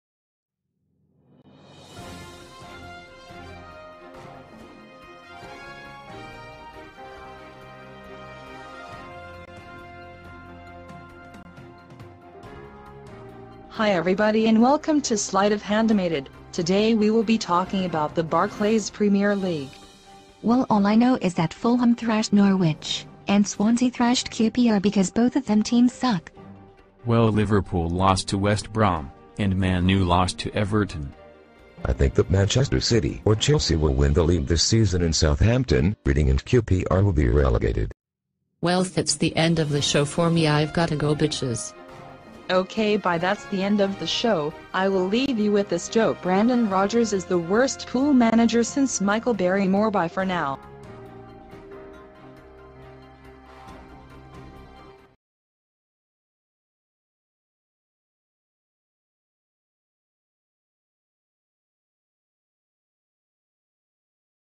slight of handimated: premiere league
this is a short 1 this week about the barclays premiere league in england